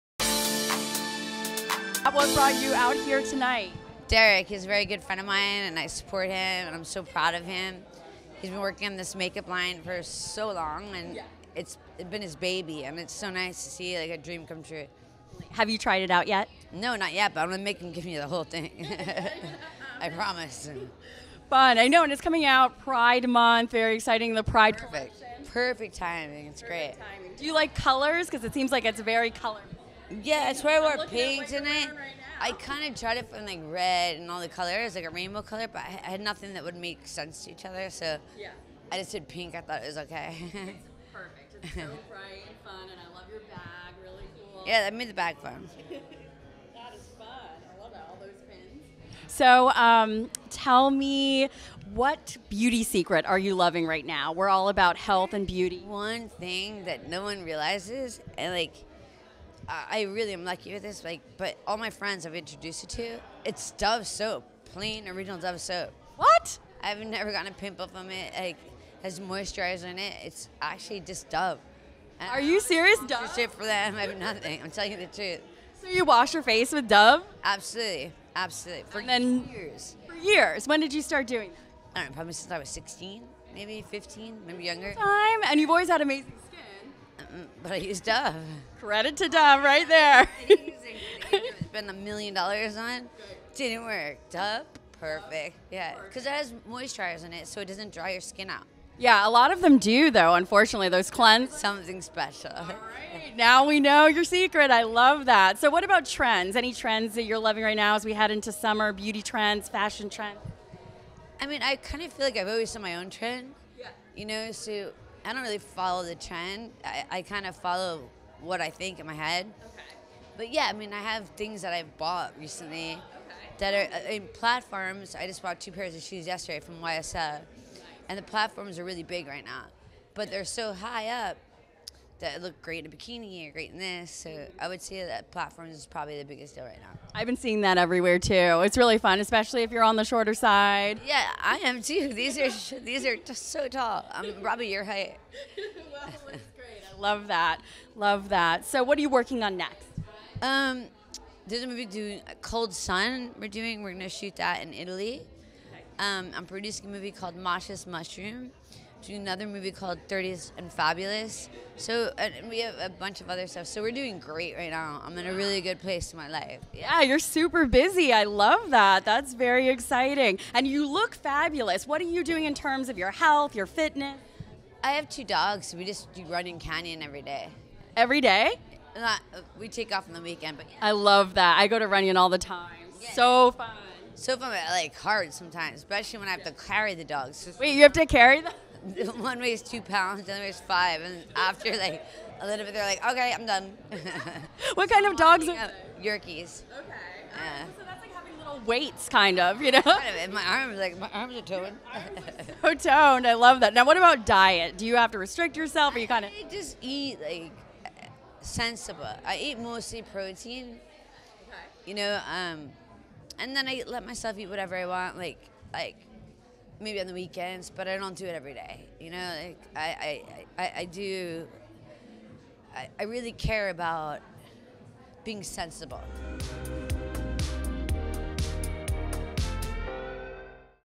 Tara Reid Celebrates the Derek Warburton x Face Stockholm Derek Fabulous Pride Collection
Actress Tara Reid reveals a special beauty secret that she includes in her everyday beauty routine. Tara Reid shares which fashion trends she is following, and what she believes is big, right now.